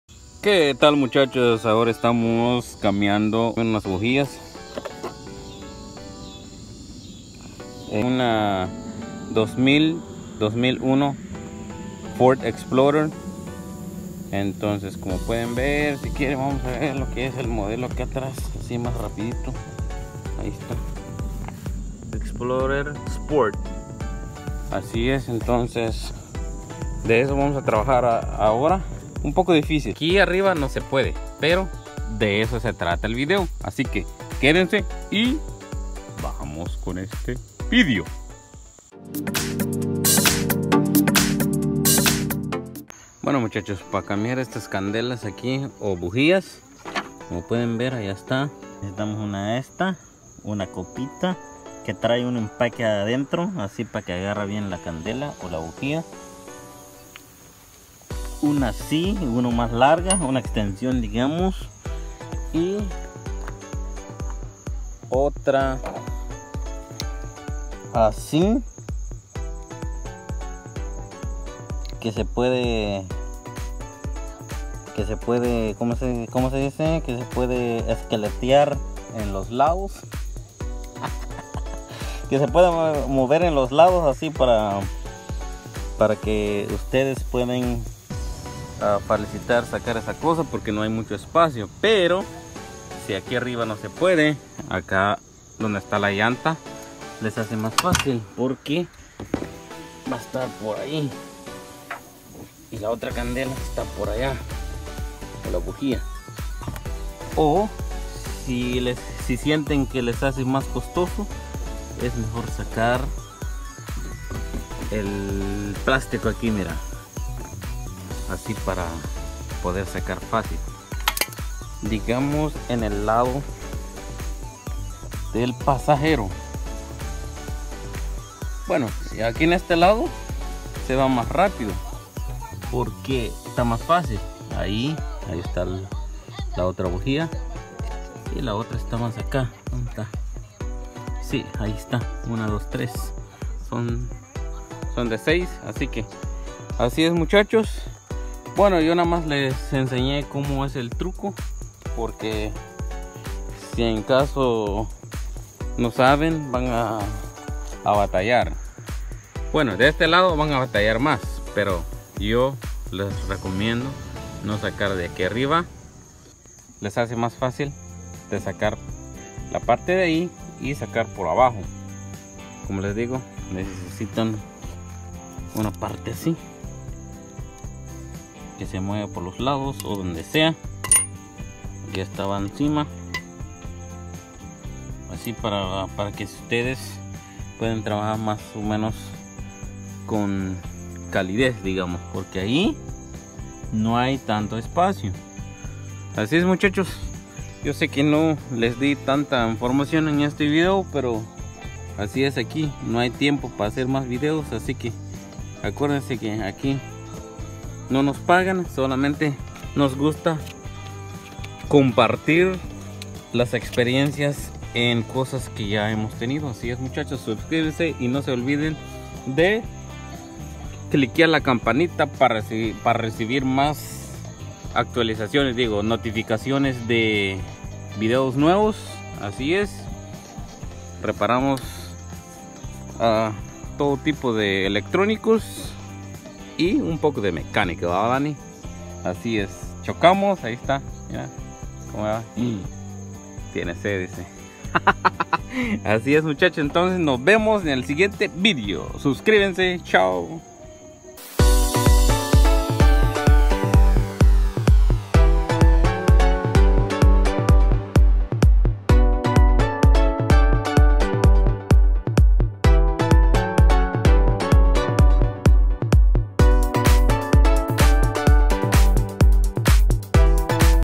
COMO CAMBIAR BUJIAS DE UN 2000 FORD EXPLORER SPORTS
Aqui cambiando las candelas o bujias de un explorer